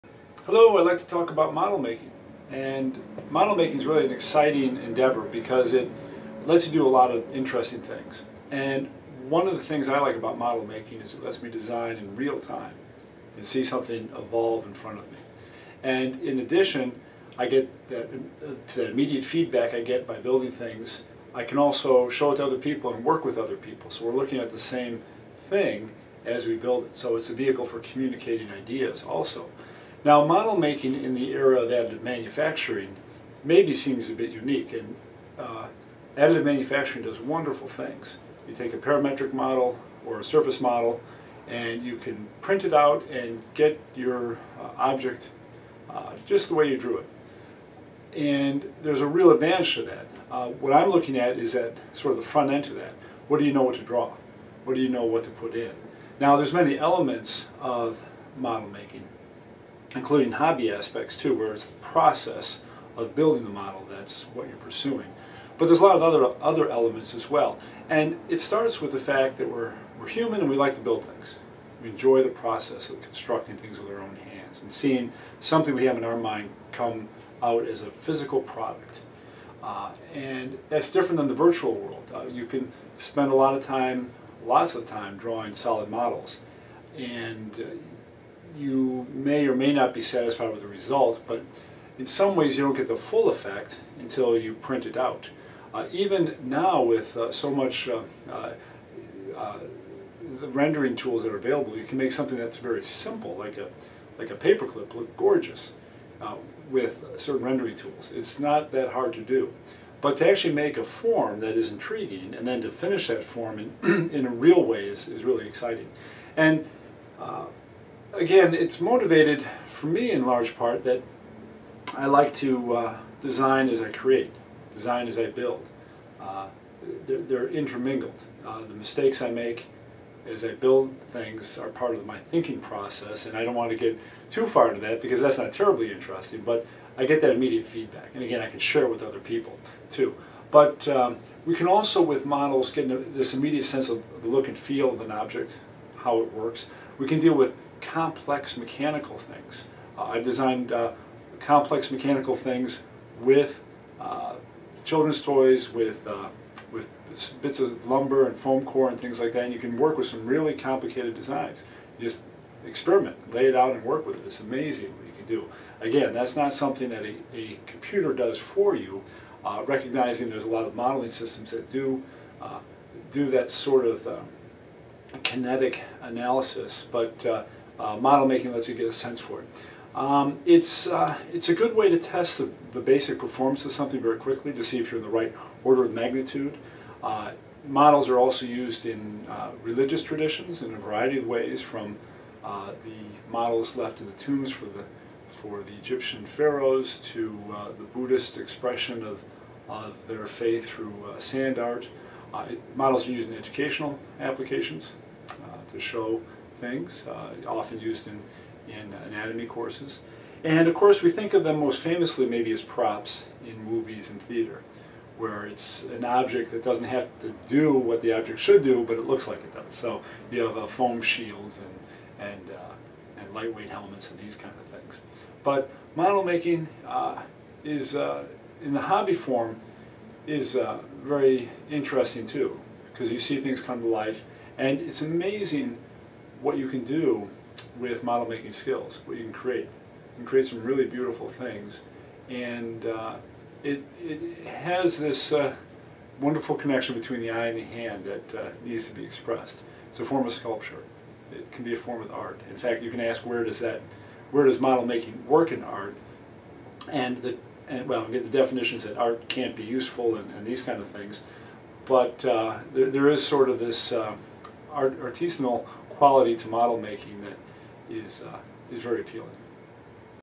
Model Building for Inventors and Designers
Context for model building in design and other applications.
Dr. Thomas Ask, professor of industrial design
Topic 12